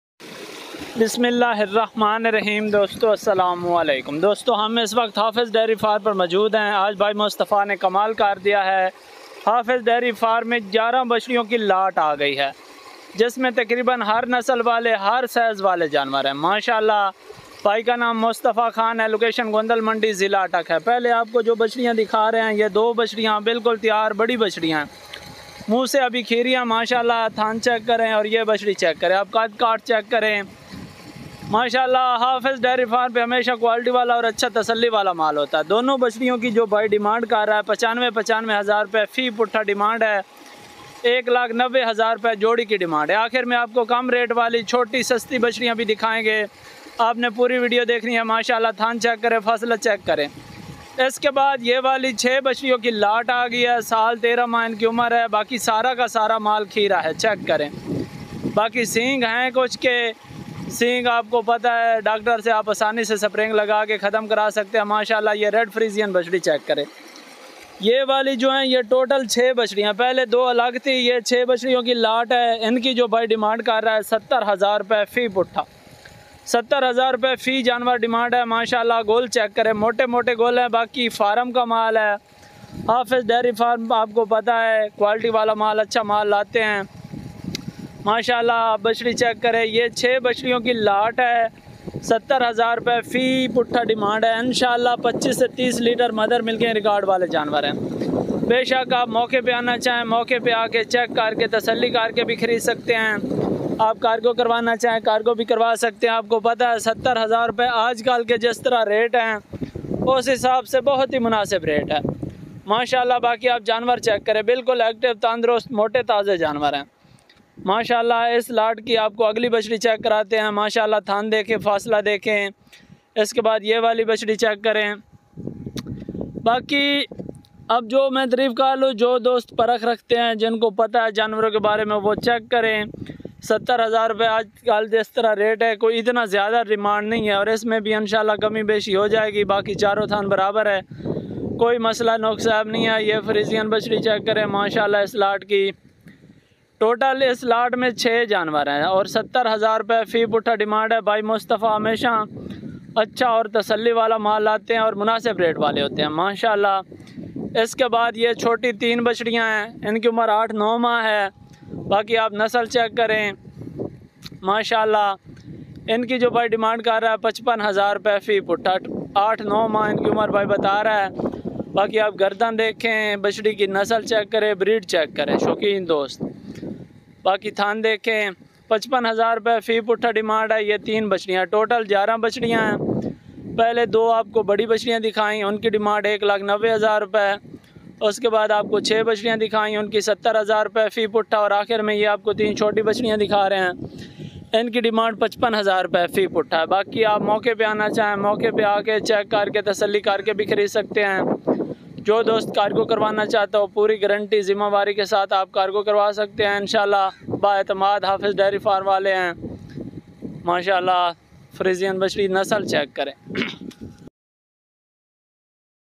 11 Red Friesian Australian Friesian Bachrian For Sale Location Hafiz Dairy Farm
11 Red Friesian Australian Friesian Bachrian For Sale Location Hafiz Dairy Farm